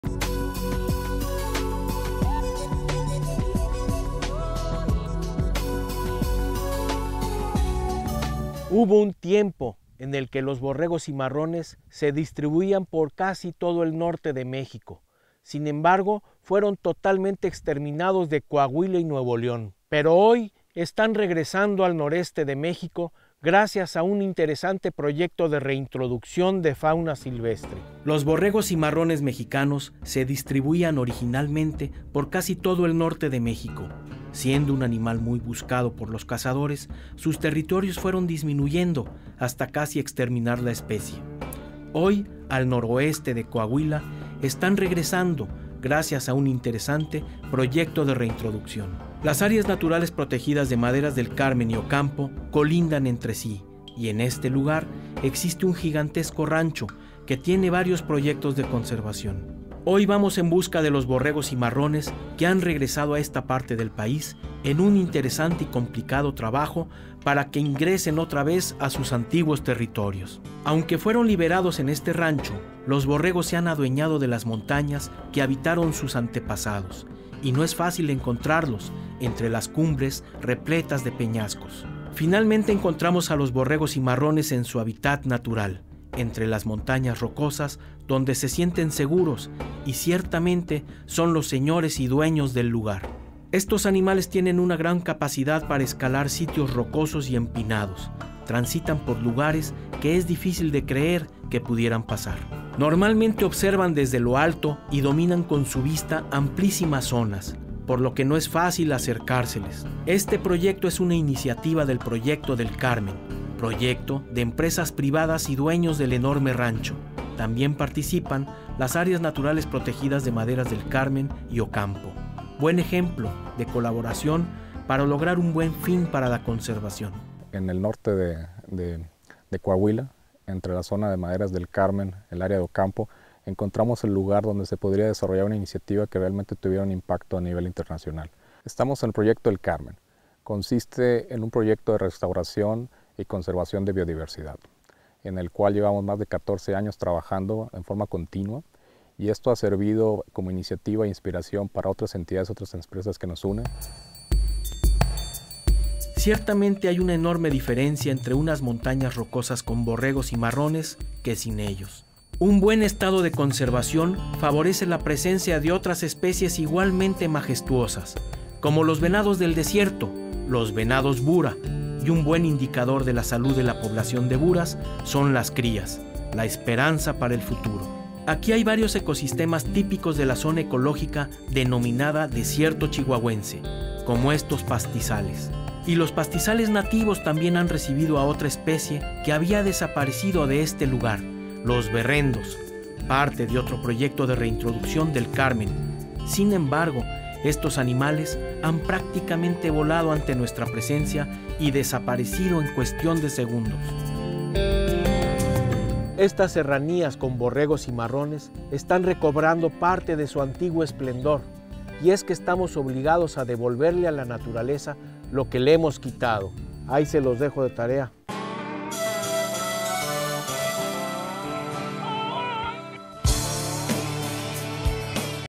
Borrrego Cimarrón, (Ovis canadensis mexicana) en Coahuila
Proyecto de Reintroducción del Borrego Cimarrón. Por Fernando Seriñá Garza